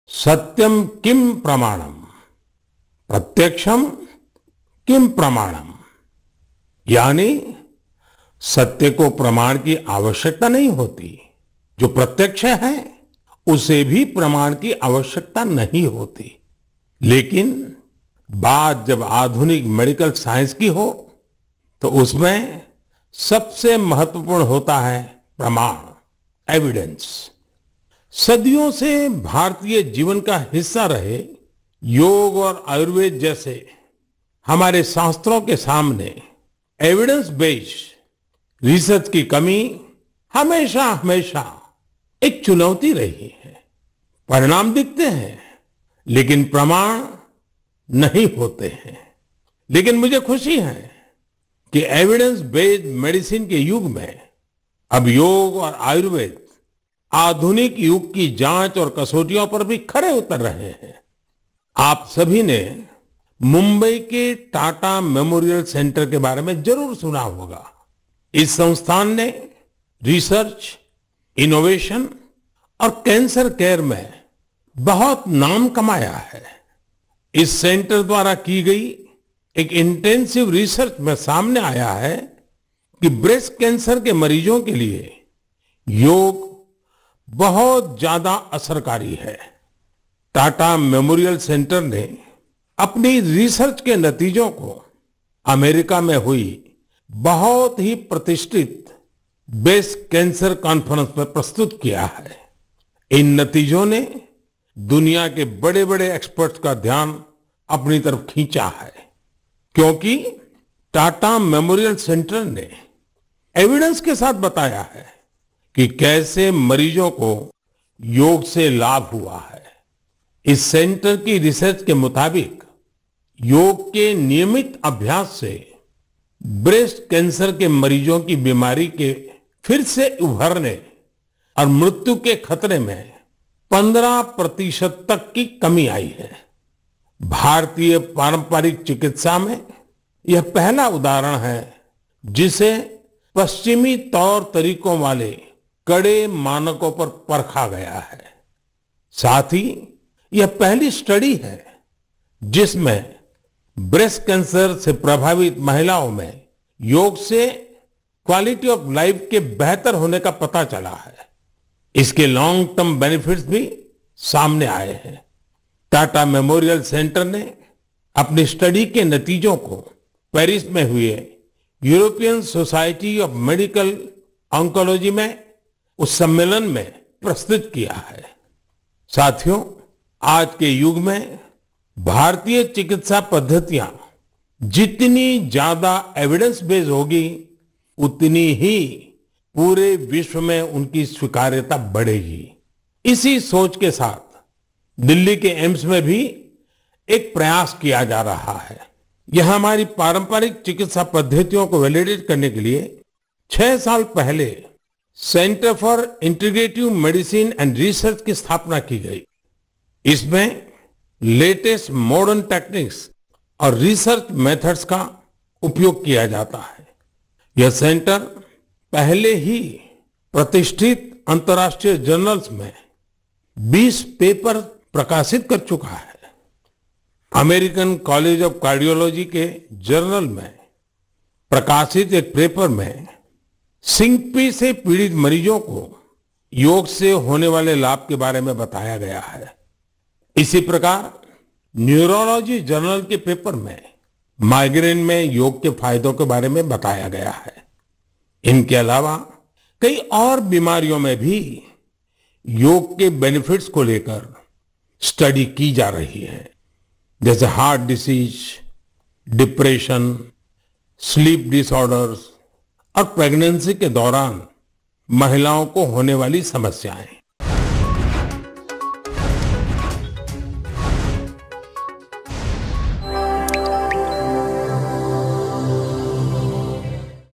ସତ୍ୟର କୈାଣସୀ ପ୍ରମାଣ ଆବଶ୍ୟକ ନାହିଁ : ପ୍ରଧାନମନ୍ତ୍ରୀ ମୋଦି
ସତ୍ୟର କୈାଣସୀ ପ୍ରମାଣ ଆବଶ୍ୟକ ନାହିଁ , ଆପଣ ଯାହା ଦେଖୁଛନ୍ତି ତାହା ମଧ୍ୟ କୈାଣସୀ ପ୍ରମାଣର ଆବଶ୍ୟକତା ନାହିଁ